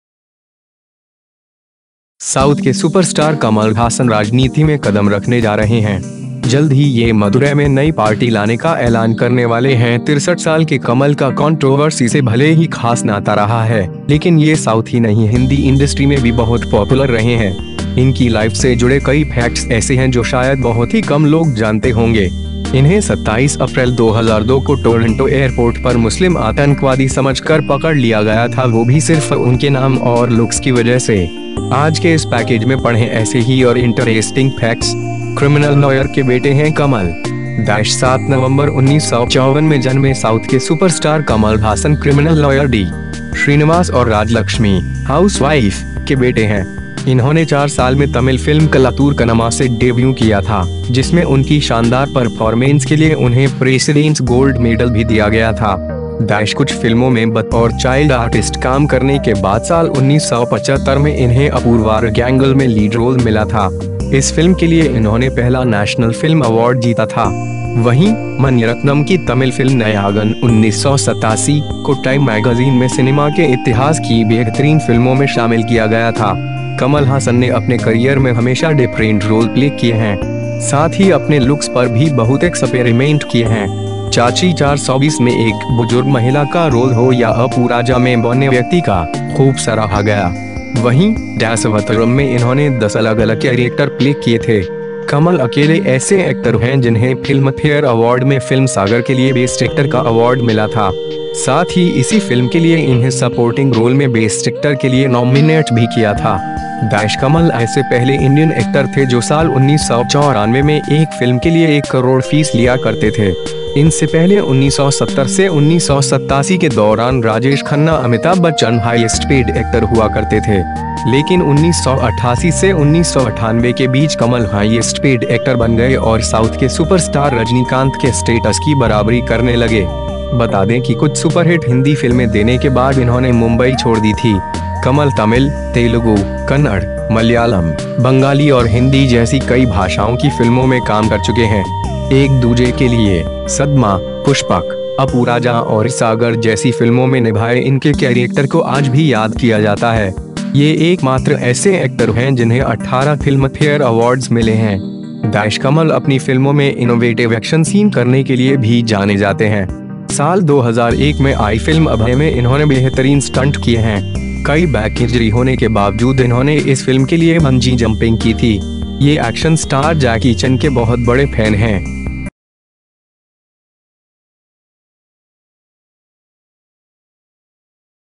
कभी टेररिस्ट समझ लिए गए थे कमल हासन, जानें उनकी लाइफ के ये फैक्ट
साउथ के सुपरस्टार कमल हासन राजनीति में कदम रखने जा रहे हैं। जल्द ही ये मदुरै में नई पार्टी लाने का ऐलान करने वाले हैं। 63 साल के कमल का कॉन्ट्रोवर्सी से भले ही खास नाता रहा है, लेकिन ये साउथ ही नहीं हिंदी इंडस्ट्री में भी बहुत पॉपुलर रहे हैं। इनकी लाइफ से जुड़े कई फैक्ट्स ऐसे हैं जो शायद बहुत ही कम लोग जानते होंगे। इन्हें 27 अप्रैल 2002 को टोरंटो एयरपोर्ट पर मुस्लिम आतंकवादी समझकर पकड़ लिया गया था वो भी सिर्फ उनके नाम और लुक्स की वजह से। आज के इस पैकेज में पढ़ें ऐसे ही और इंटरेस्टिंग फैक्ट्स। क्रिमिनल लॉयर के बेटे हैं कमल...

- 7 नवंबर 1954 में जन्मे साउथ के सुपरस्टार कमल हासन क्रिमिनल लॉयर डी. श्रीनिवास और राजलक्ष्मी (हाउसवाइफ) के बेटे हैं। इन्होंने 4 साल में तमिल फिल्म 'कलत्तूर कन्नम्मा' से डेब्यू किया था, जिसमें उनकी शानदार परफॉर्मेंस के लिए उन्हें प्रेसीडेंट्स गोल्ड मैडल भी दिया गया था।

- कुछ फिल्मों में बतौर चाइल्ड आर्टिस्ट काम करने के बाद साल 1975 में इन्हें 'Apoorva Raagangal' में लीड रोल मिला था। इस फिल्म के लिए इन्होंने पहला नेशनल फिल्म अवॉर्ड जीता था। वहीं, मणिरत्नम की तमिल फिल्म 'Nayagan' (1987) को टाइम मैगजीन में सिनेमा के इतिहास की बेहतरीन फिल्मों में शामिल किया गया था।

 कमल हासन ने अपने करियर में हमेशा डिफरेंट रोल प्ले किए हैं। साथ ही अपने लुक्स पर भी बहुत एक्सपेरिमेंट किए हैं। 'चाची 420' में एक बुजुर्ग महिला का रोल हो या 'अप्पू राजा' में बौने व्यक्ति का, खूब सराहा गया। वहीं, 'Dasavathaaram' में इन्होंने 10 अलग-अलग कैरेक्टर प्ले किए थे। कमल अकेले ऐसे एक्टर हैं जिन्हें फिल्मफेयर अवॉर्ड में फिल्म 'सागर' के लिए बेस्ट एक्टर का अवॉर्ड मिला था। साथ ही इसी फिल्म के लिए इन्हें सपोर्टिंग रोल में बेस्ट एक्टर के लिए नॉमिनेट भी किया था।

- कमल ऐसे पहले इंडियन एक्टर थे जो साल 1994 में एक फिल्म के लिए 1 करोड़ फीस लिया करते थे। इनसे पहले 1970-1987 के दौरान राजेश खन्ना-अमिताभ बच्चन हाइएस्ट पेड एक्टर हुआ करते थे। लेकिन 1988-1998 के बीच कमल हाइएस्ट पेड एक्टर बन गए और साउथ के सुपरस्टार रजनीकांत के स्टेटस की बराबरी करने लगे। बता दें कि कुछ सुपरहिट हिंदी फिल्में देने के बाद इन्होंने मुंबई छोड़ दी थी।

कमल तमिल, तेलुगु, कन्नड़, मलयालम, बंगाली और हिंदी जैसी कई भाषाओं की फिल्मों में काम कर चुके हैं। 'एक दूजे के लिए', 'सदमा', 'पुष्पक', 'अप्पू राजा' और 'सागर' जैसी फिल्मों में निभाए इनके कैरेक्टर को आज भी याद किया जाता है। ये एकमात्र ऐसे एक्टर हैं जिन्हें 18 फिल्मफेयर अवॉर्ड्स मिले हैं।

- कमल अपनी फिल्मों में इनोवेटिव एक्शन सीन करने के लिए भी जाने जाते हैं। साल 2001 में आई फिल्म 'अभय' में इन्होंने बेहतरीन स्टंट किए हैं। कई बैक इंजरी होने के बावजूद इन्होंने इस फिल्म के लिए बंजी जंपिंग की थी। ये एक्शन स्टार जैकी चैन के बहुत बड़े फैन हैं।